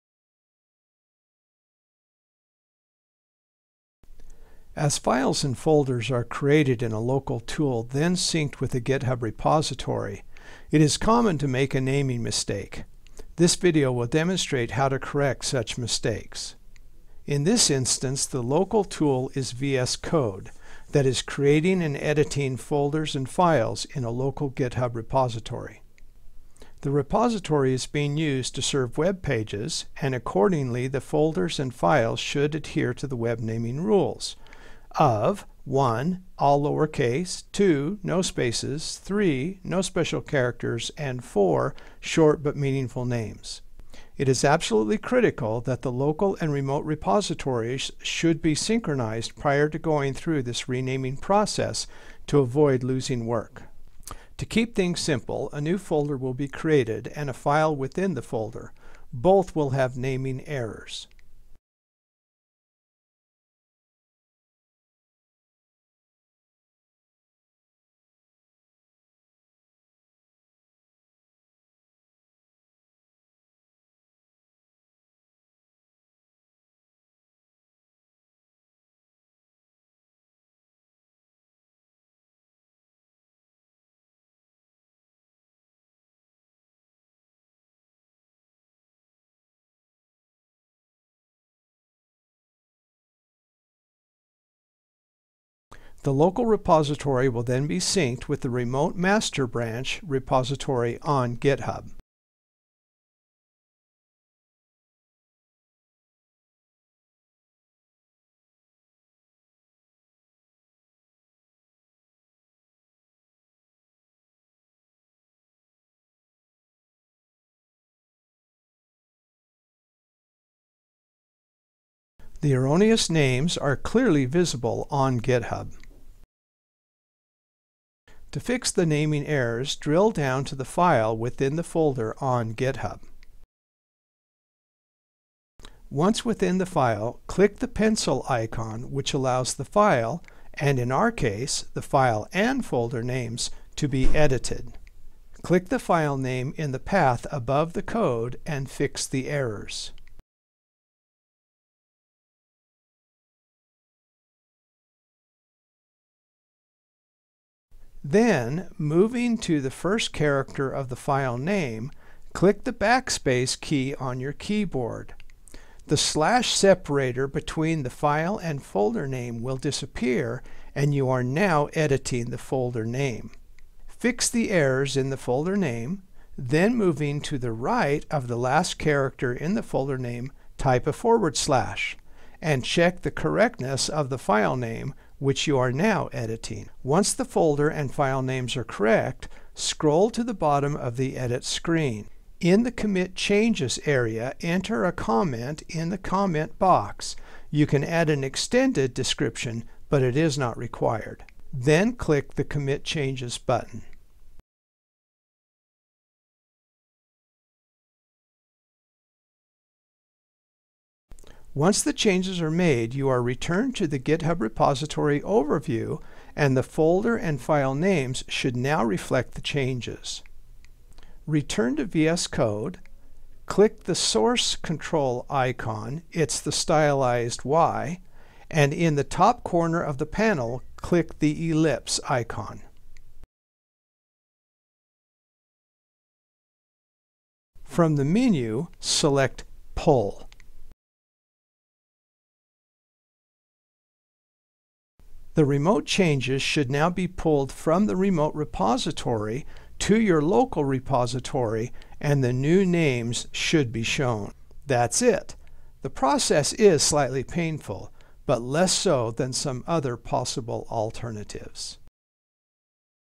Renaming folder and file names on github
Making a naming mistake locally and pushing it to github and then fixing it is the topic of this video. The process involves VS code working with a local web repository and the remote repository on github.